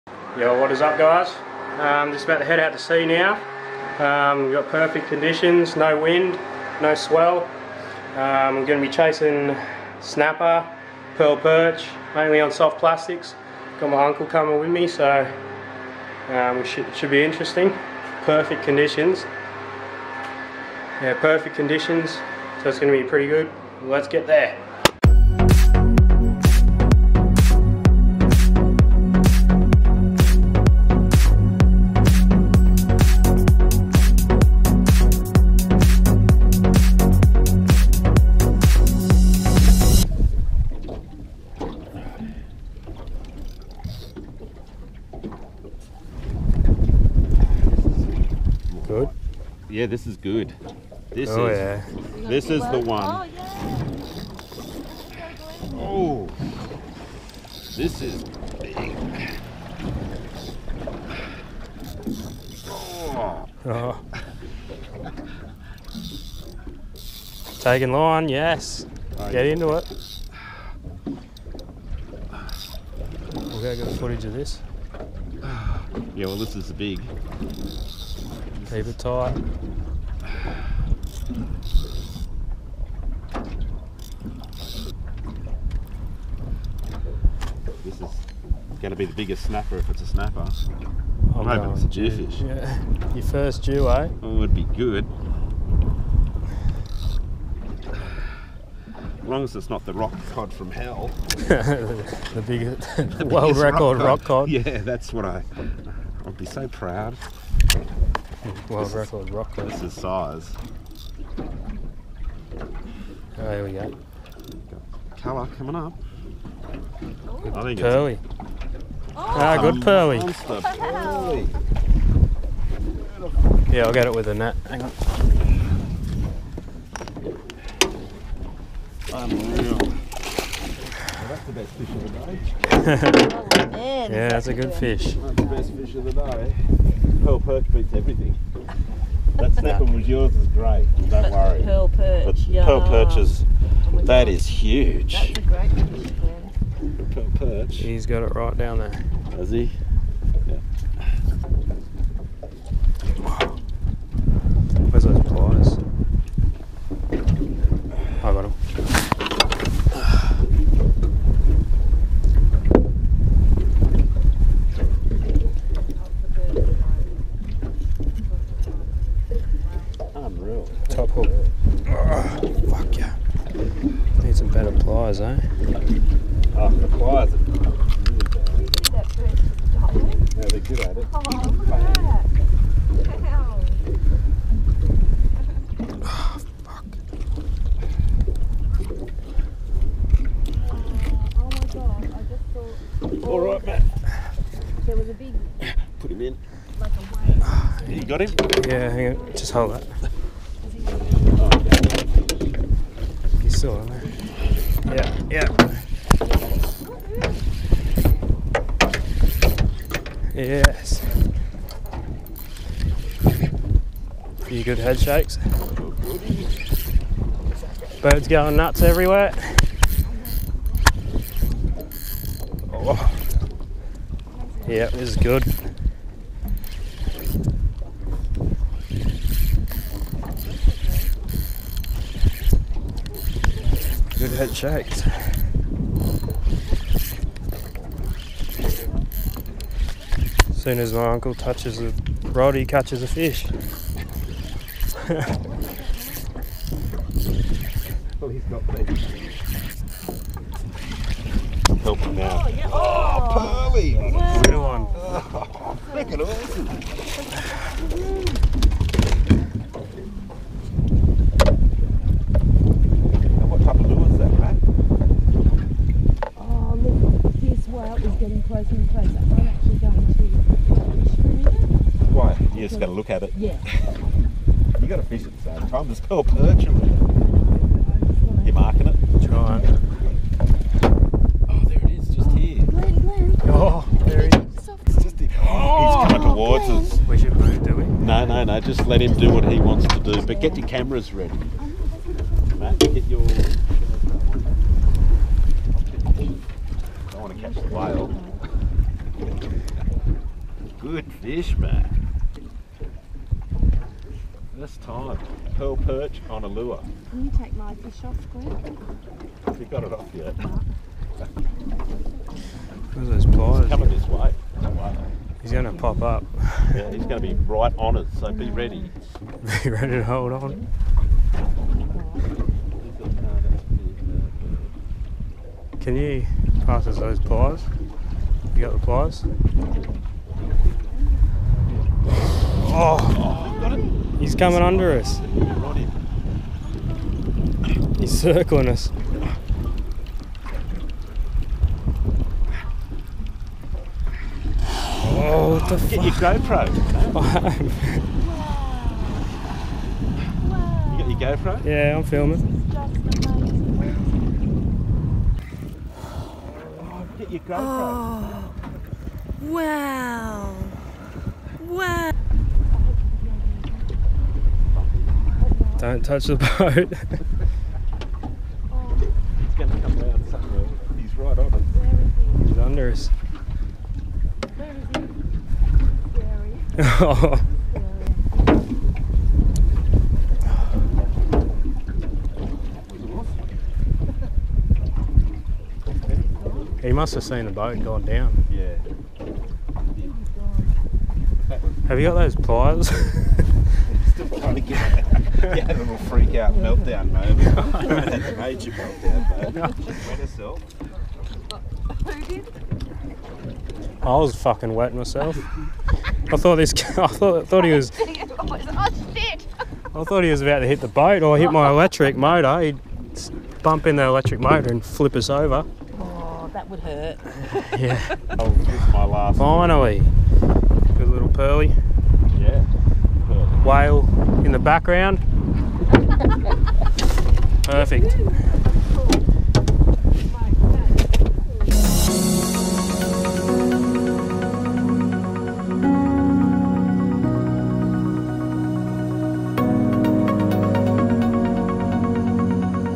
A WHALE NEARLY FLIPPED MY BOAT WHILE FISHING
new video today is from a fishing trip the other day while we were fishing for pearl perch a whale decided to come up to my boat please give this video a thumbs up and a sub too would be nice thinks